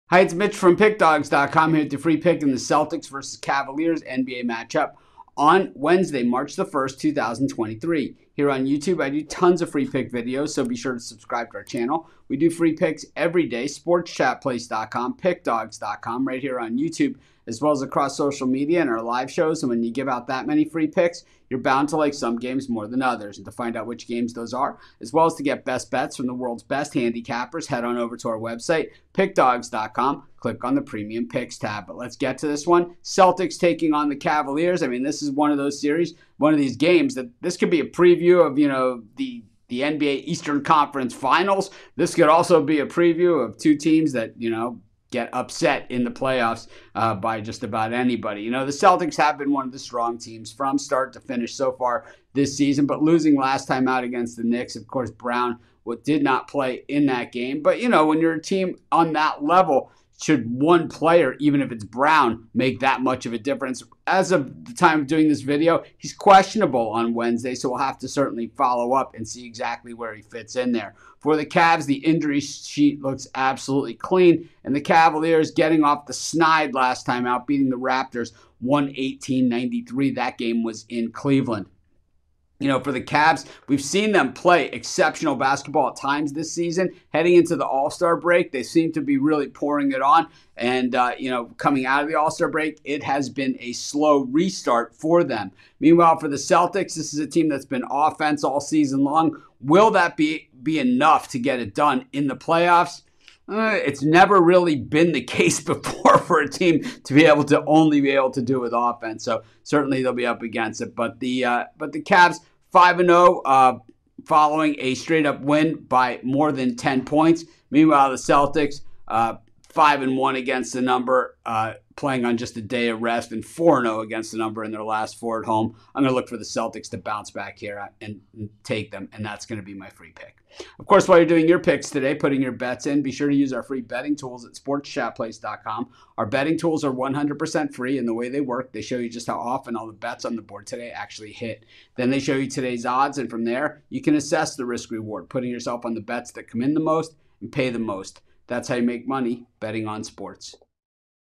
Boston Celtics vs Cleveland Cavaliers 3/1/23 NBA Free Picks & Predictions | NBA Betting Tips
The Cleveland Cavaliers face the Boston Celtics in Wednesday's NBA Basketball action. Get Mitch's NBA Pick and Prediction for this game.